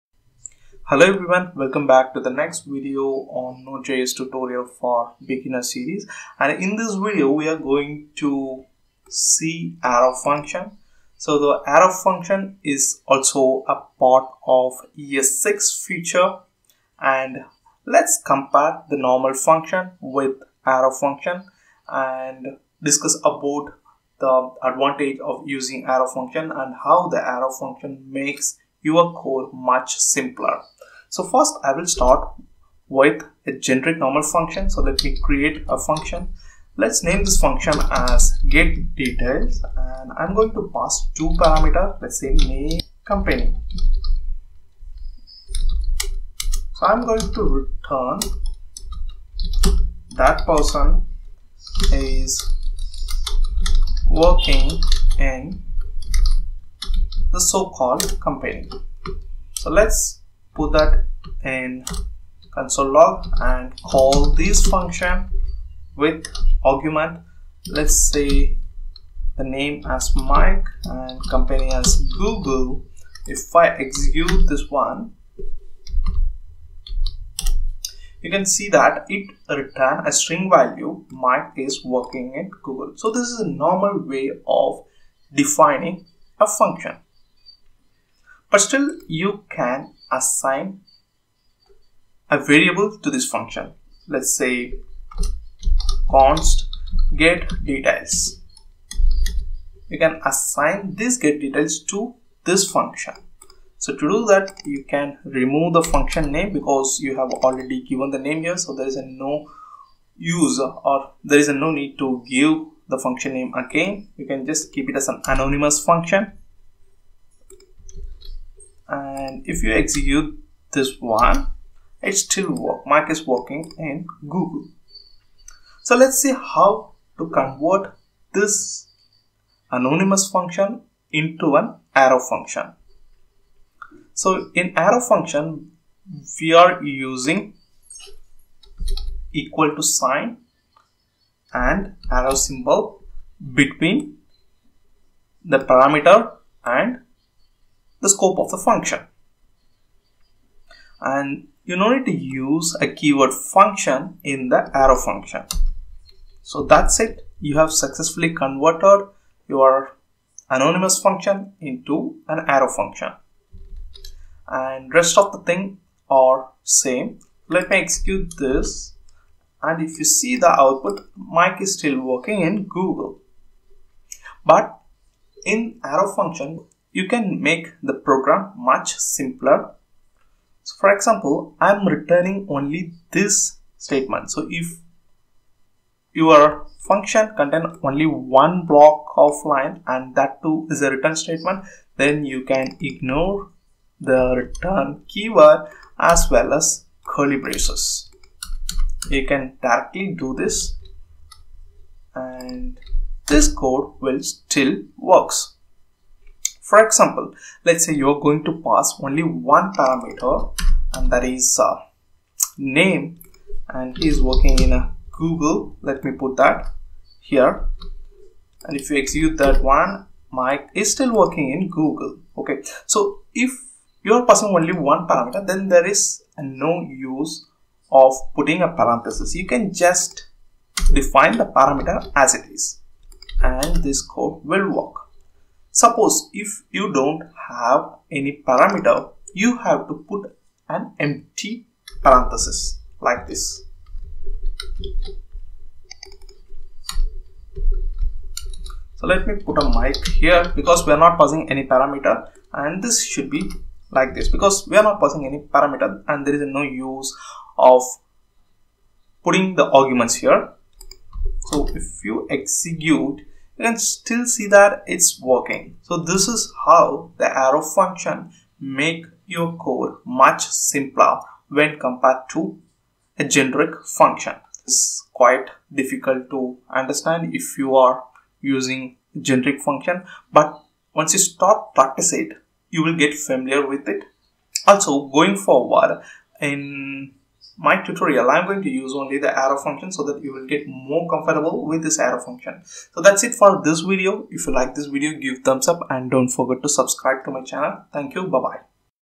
Arrow Function Javascript - Node JS tutorial for beginners
Arrow function in javascript was introduced in ES6 version. It makes the syntax looks very simple and short with so many features incorporated.

For example, if the arrow function in javascript has only one statement and you no need to use the return statement. By default, it returns the value.

Just have a look at the above video on how the arrow function works in javascript.

Autoit tutorials for beginners
Excel VBA for beginners

_Audio & Microphone